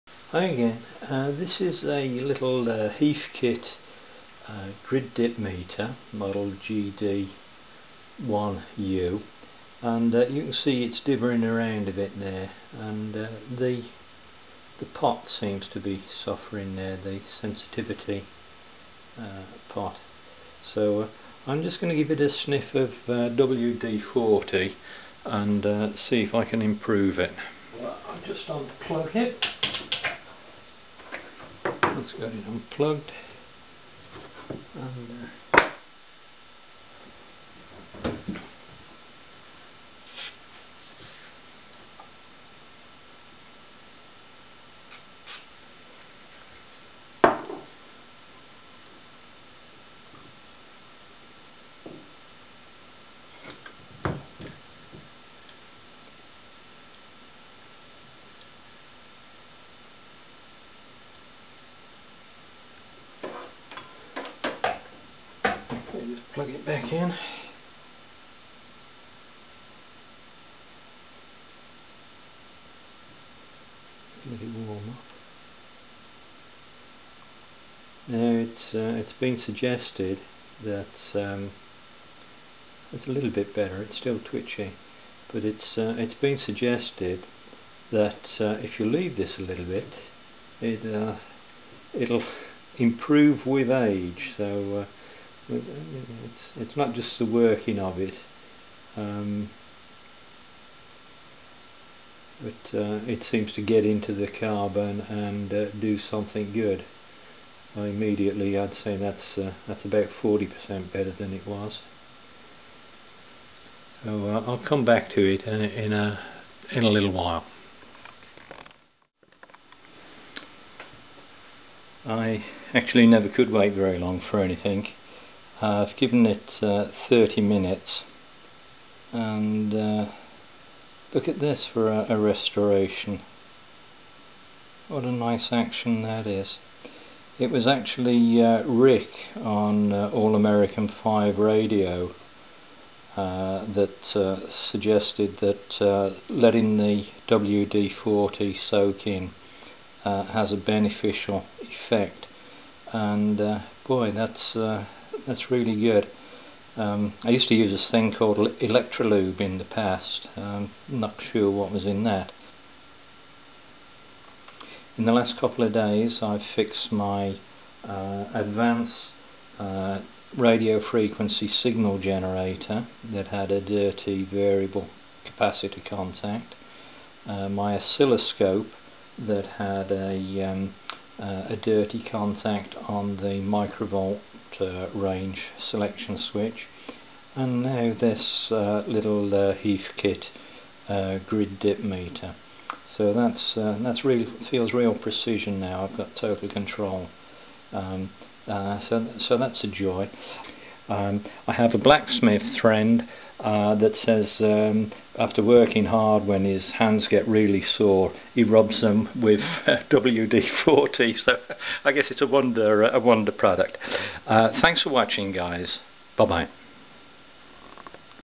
Grid Dip Oscillator, fixing a dirty pot.
In this video I show how you can use WD-40 to fix a dirty potentiometer, but note that the beneficial results improve with time.